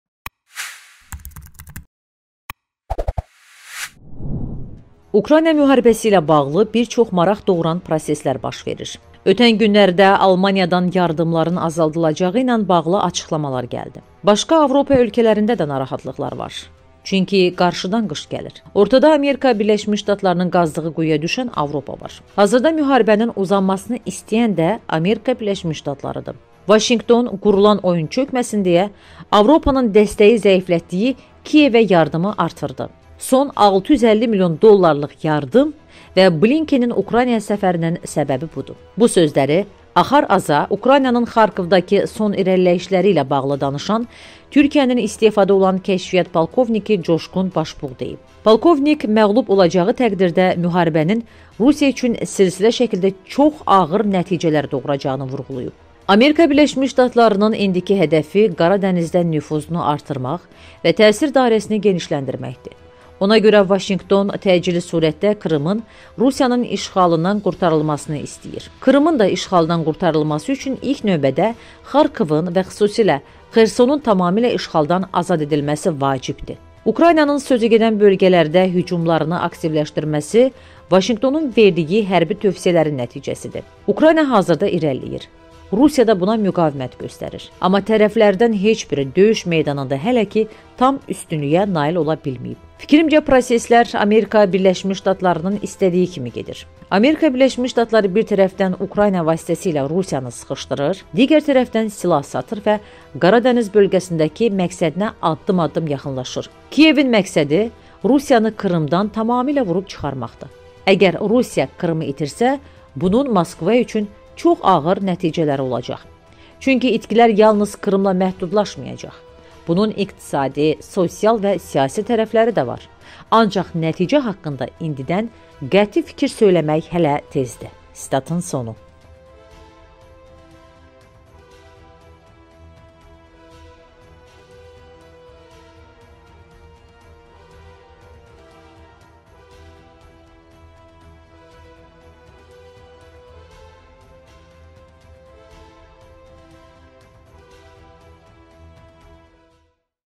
UKRAYNANIN HÜCUM PLANINI BU ÖLKƏ CIZIB - BAŞBUĞ
Ukrayna müharibəsi ilə bağlı bir çox maraq doğuran proseslər baş verir. Ötən günlərdə Almaniyadan yardımların azaldılacağı ilə bağlı açıqlamalar gəldi. Başqa Avropa ölkələrində də narahatlıqlar var. Çünki qarşıdan qış gəlir. Ortada ABŞ-ın qazdığı quyuya düşən Avropa var. Hazırda müharibənin uzanmasını istəyən də ABŞ-dır. Vaşinqton qurulan oyun çökməsin deyə və Avropanın dəstəyi zəiflətdiyi Kiyevə yardımı artırdı. Son 650 milyon dollarlıq yardım və Blinkenin Ukraynaya səfərinin səbəbi budur.